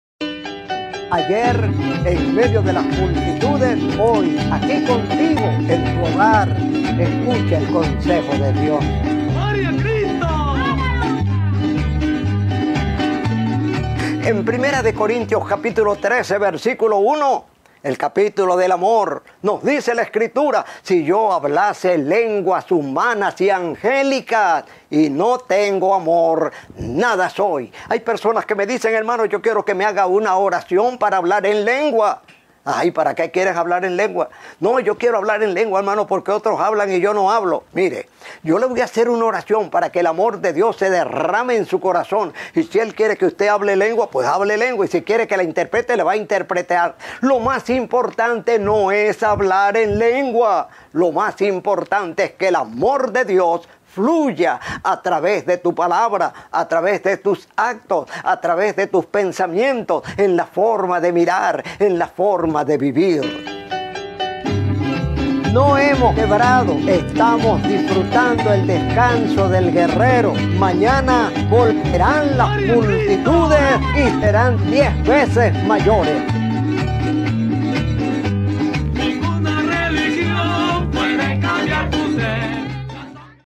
HNO PUERTAS #396 | EL AMOR | PRÉDICA BÍBLICA | MICRO HNO PUERTAS | LUZ TV
Lo mas importante es que el amor de Dios fluya a través de ti.

pastor Jaime Puertas:
El Amor
1 Corintios 13.1